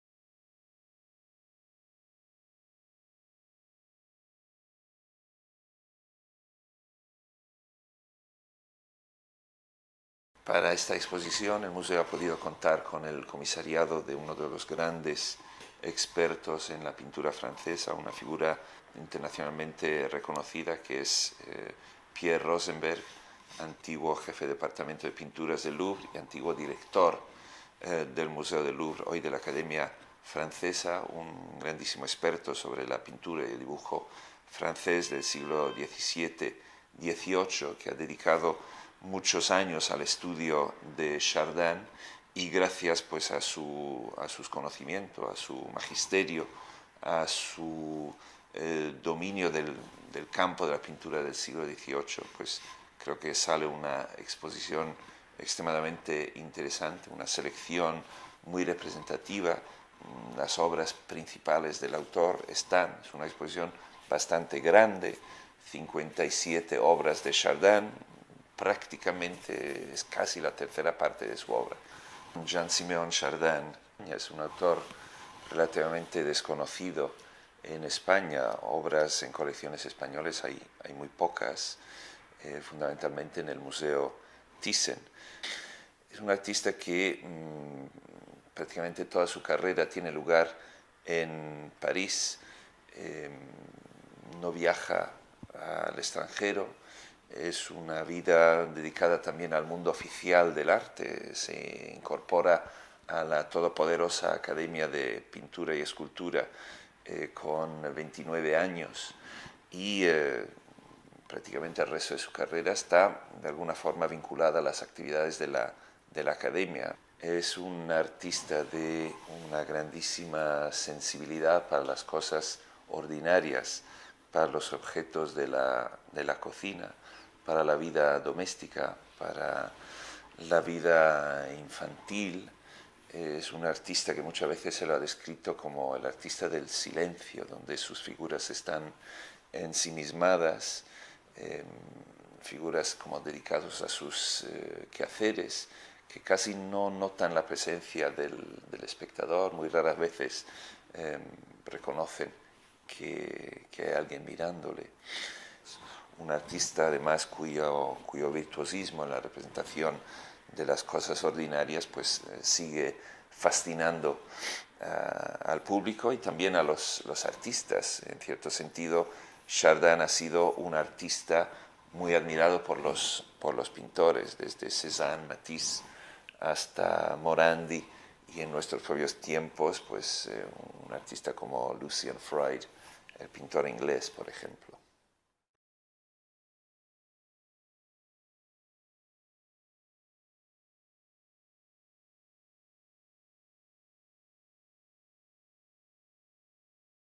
Avance de Exposición: Chardin (1699 - 1779)
Avance en video de la exposición Chardin (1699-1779) que se presenta en el Prado del 1 de marzo al 29 de mayo de 2011. Explicaciones a cargo de Gabriele Finaldi, Director Adjunto de Conservación e Investigación del Museo del Prado.

Tras las exposiciones dedicadas a Chardin con motivo del bicentenario de la muerte del artista y el tricentenario de su nacimiento, en 1979 y 1999, no se había vuelto a revisar la relativamente escasa producción -algo más de doscientas obras- de este admirado y original pintor. Con 57 obras, la muestra constituye una oportunidad excepcional para dar a conocer su obra por primera vez en España. El recorrido de la exposición sigue un orden cronológico atravesando los aspectos más destacados desde los inicios de la vida artística de Chardin, en la segunda década del siglo XVIII, hasta los pasteles de los años setenta.

Advance information on the exhibition Chardin (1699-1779) with comments by Gabriele Finaldi, Associate Director of Curatorship and Research at the Museo Nacional del Prado.

Since the exhibitions on Chardin organised in conjunction with the bicentenary of his death and the tercentenary of his birth, in 1979 and 1999 respectively, there have been no further revisions of the relatively small oeuvre (around 200 works) of this admired and highly original artist. Featuring 57 paintings, the present exhibition offers a rare opportunity to appreciate Chardin’s work and is the first on the artist to be held in Spain. The exhibition is structured chronologically, covering the most important phases of the artist’s career from his beginnings in the second decade of the 18th century to his late pastels of the 1770s.

With spanish and english subtitles